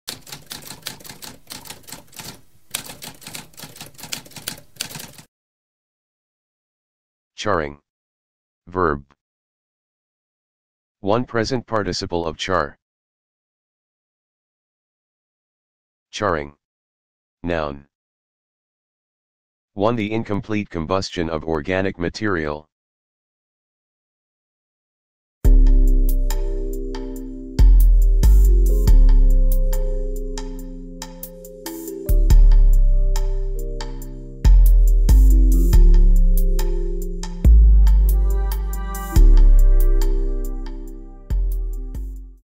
What does charring mean?
A spoken definition of charring.

Intro Sound:
Typewriter - Tamskp